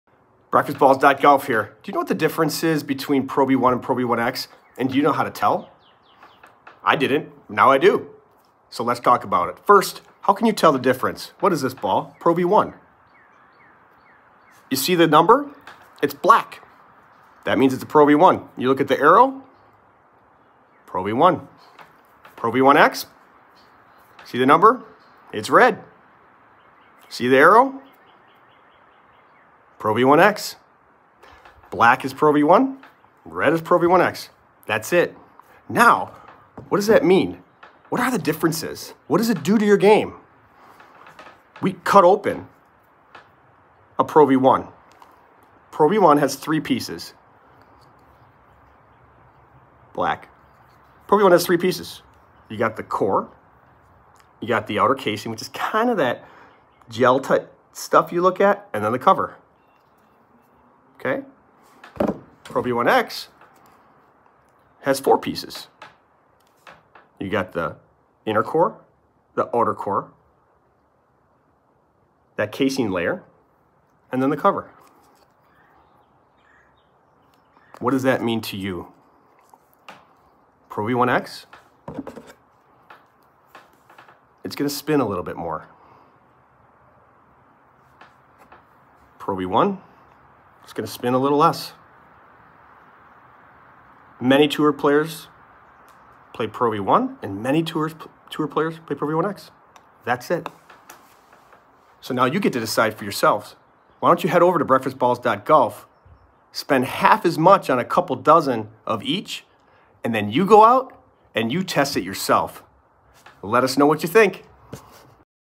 Difference between Titleist ProV1 and ProV1X
explain the difference between the new Titleist ProV1 and ProV1X golf balls!  Look inside each ball and see which is right for your golf game.

for more detail in our blog!